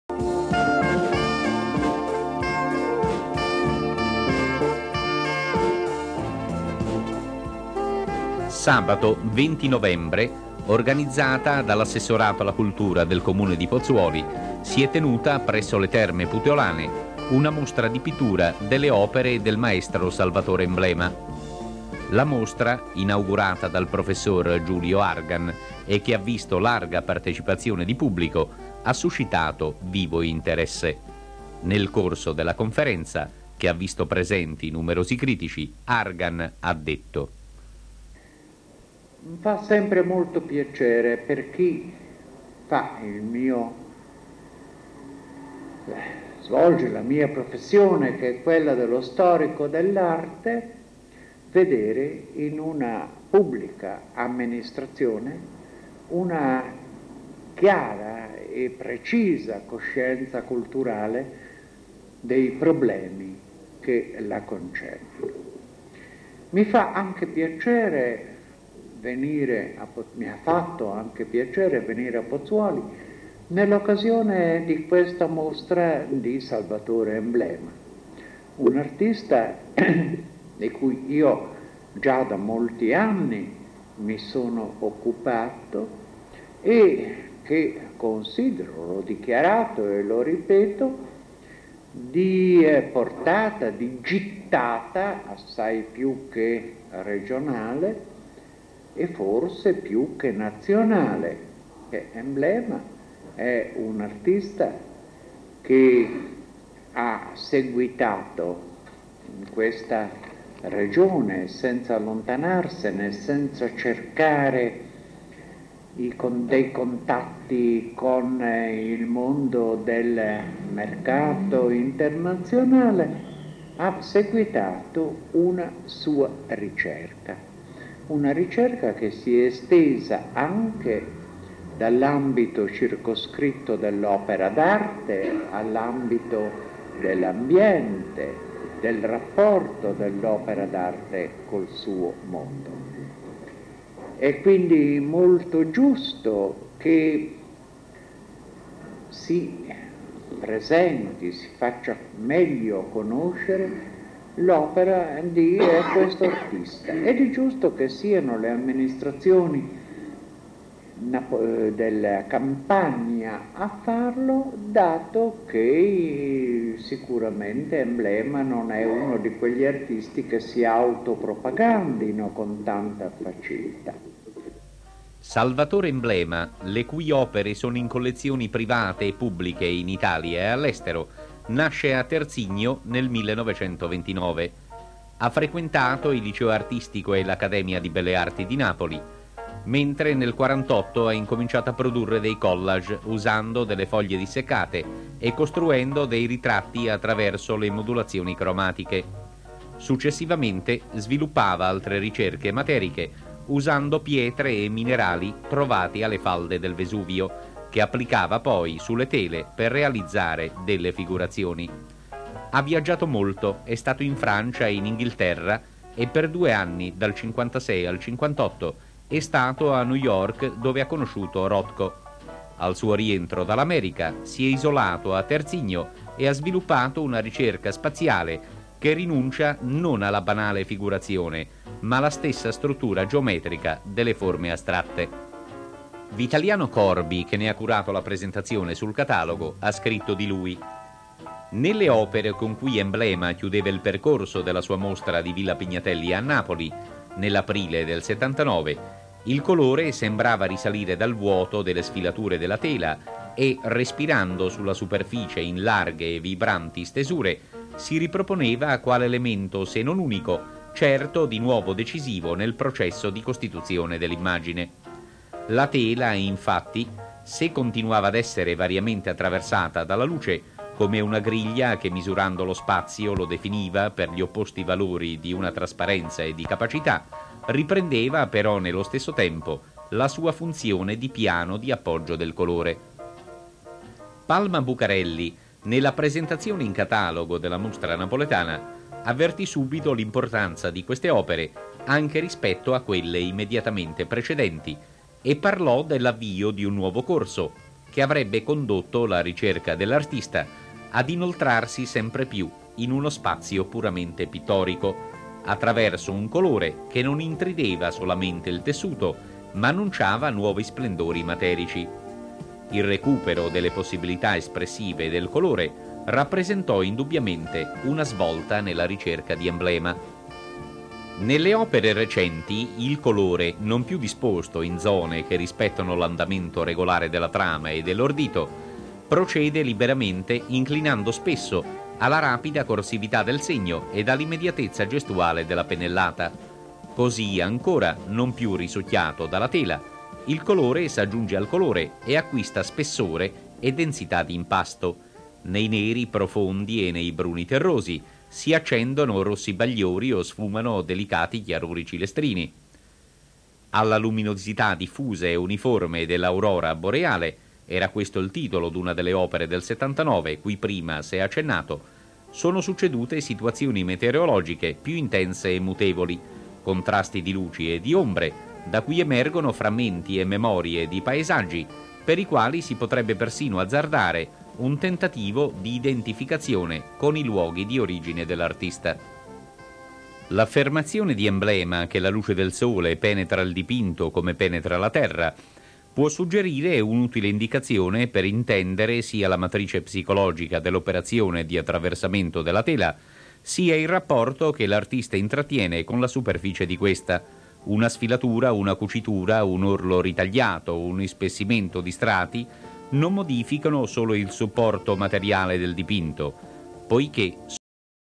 SALVATORE EMBLEMA conferenza G.C.Argan
Mostra personale tenuta nel 1982 alle Terme Puteolane, Pozzuoli. Preceduta dalla conferenza di Giulio Carlo Argan